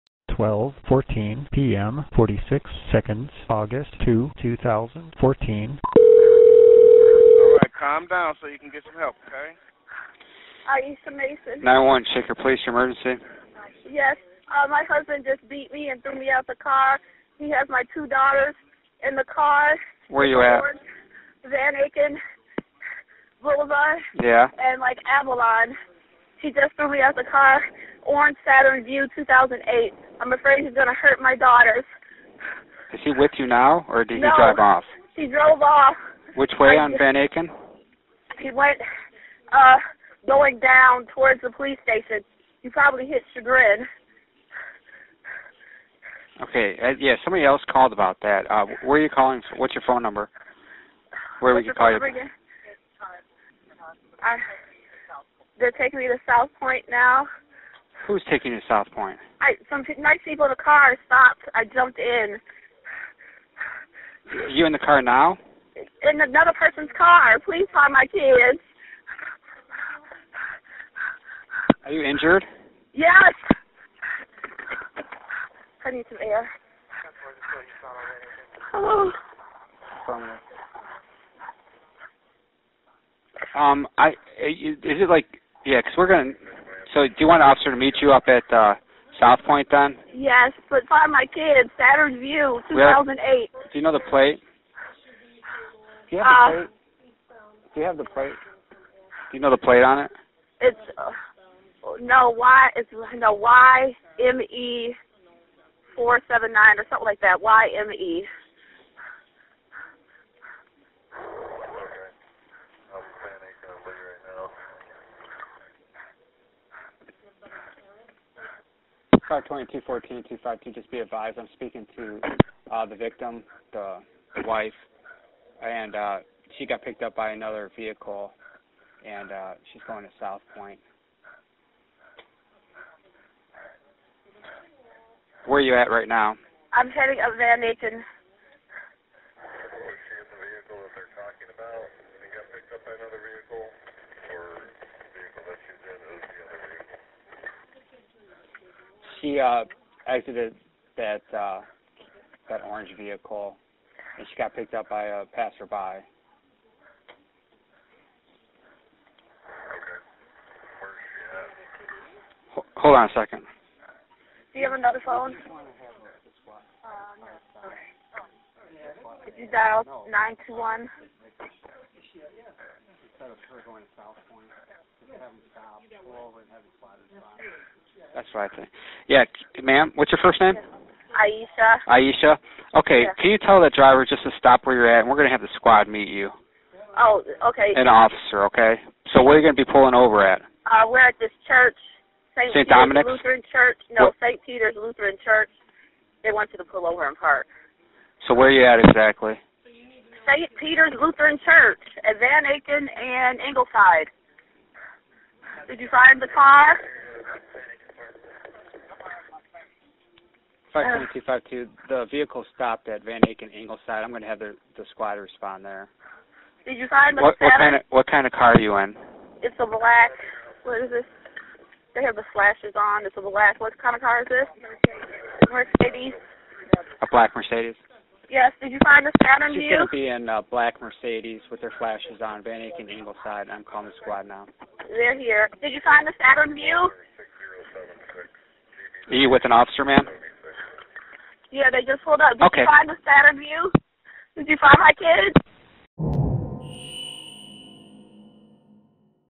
Lance Mason beating incident 911 call
Former judge Lance Mason, who was forced to resign from the Cuyahoga County Court of Common Pleas after beating his wife, has a new job at Cleveland City Hall.